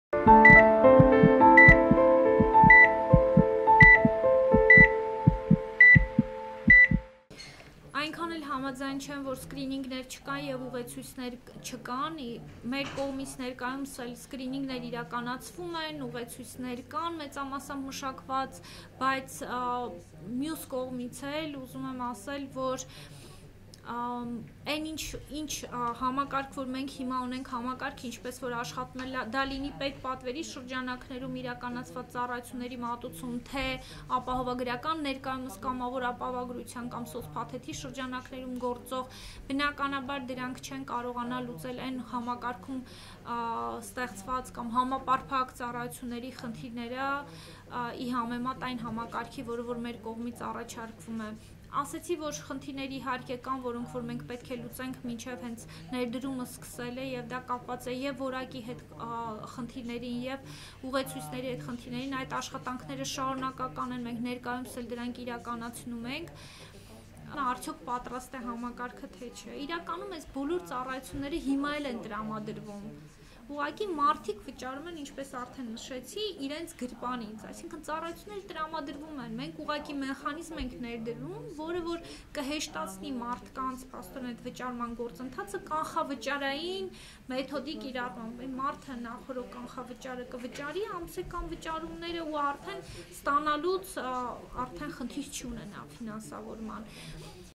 Աշխատավարձի 6 %-ը վերցնում են և առաջարկում առողջապահական ապահովագրություն, թե՞ ինչպես դեռ որոշված չէ
Առողջապահության նախարարությունը նախատեսում է կիրառել 6 % պարտադիր պետական առողջապահական հարկ։ Հարկատուներին, դրա համար վճարող 600 հազար աշխատող քաղաքացիներին հետաքրքրում է արդո՞ք մեր առողջապահական համակարգը պատրաստ է նման համակարգի ներդրմանը։ Առողջության պահպանման առաջնային օղակը՝ պոլիկլիլնիկաները,  կարող է սպասերկել բնակչությանը հավուր պատշաճի։